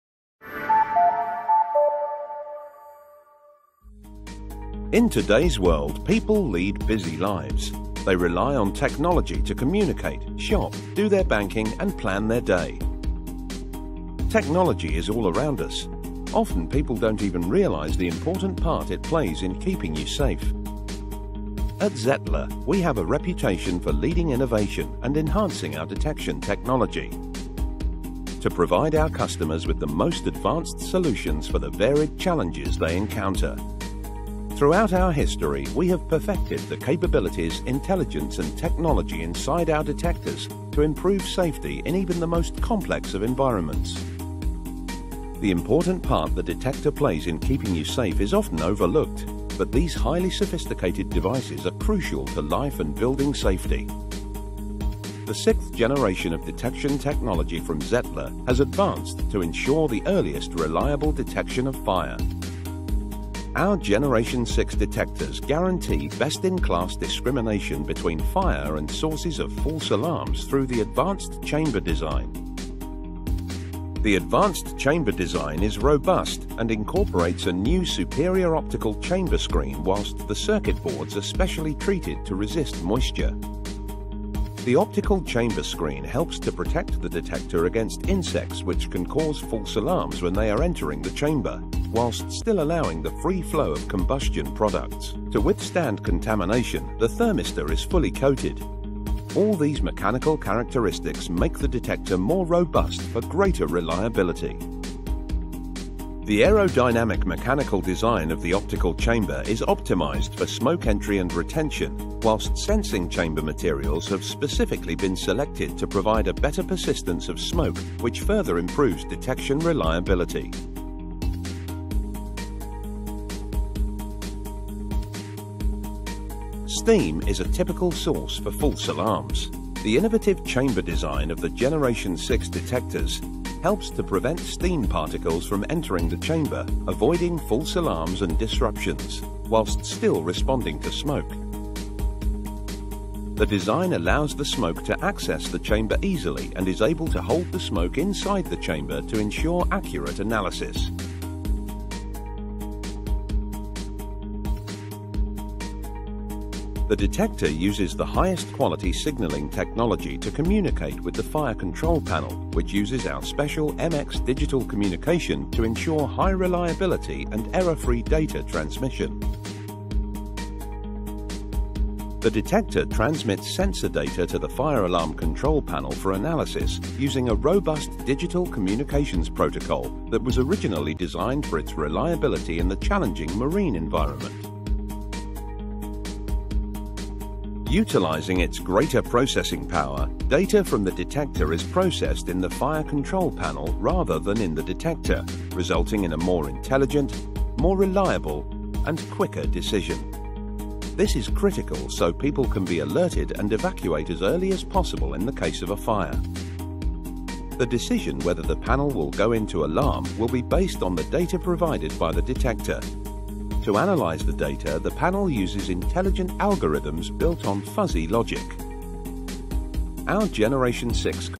ZETTLER Generation 6 Fire Detection Technology
Fire detection technology from ZETTLER is designed to reduce false alarms. We have enhanced the design of the chamber to help achieve a reduction in false alarms. Watch the video and learn how it works.